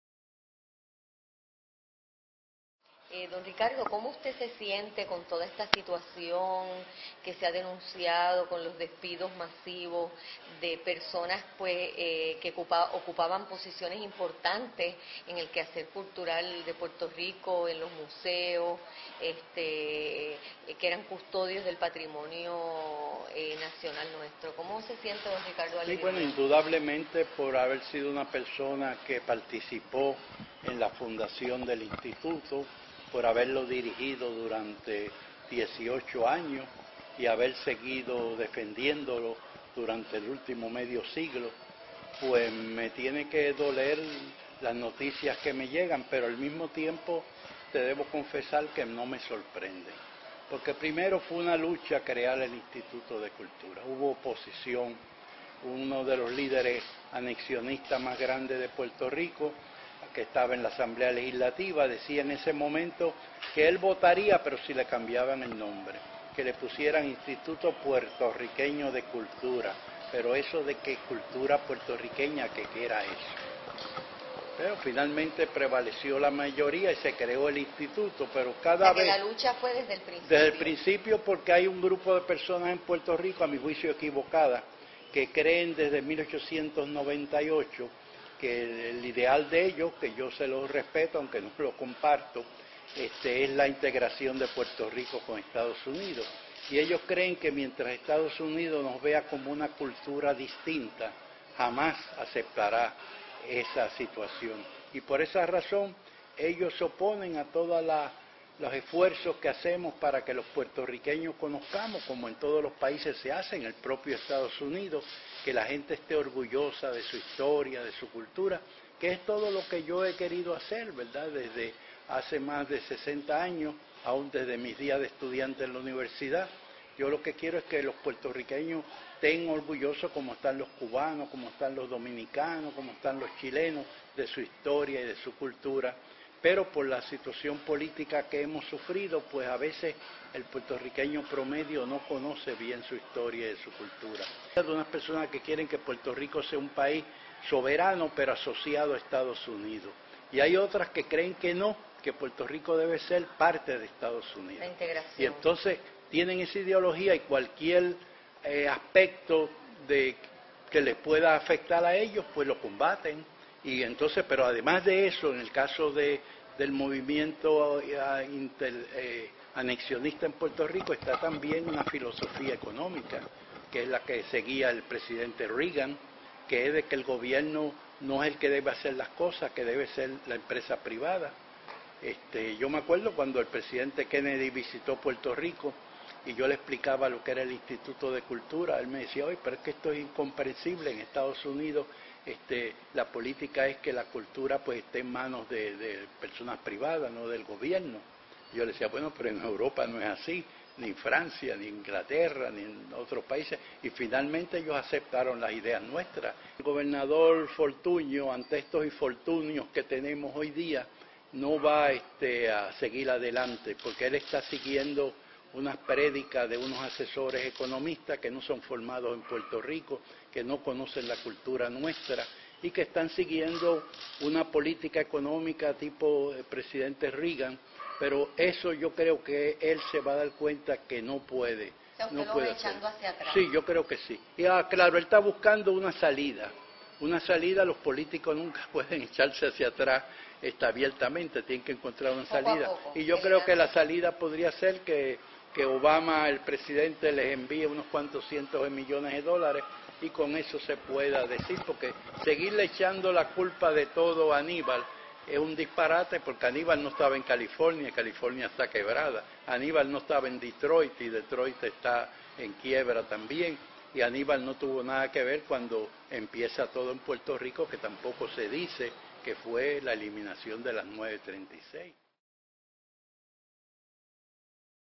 En defensa de la cultura
Don Ricardo Alegría opina sobre los despidos en el Instituto de Cultura Puertorriqueña.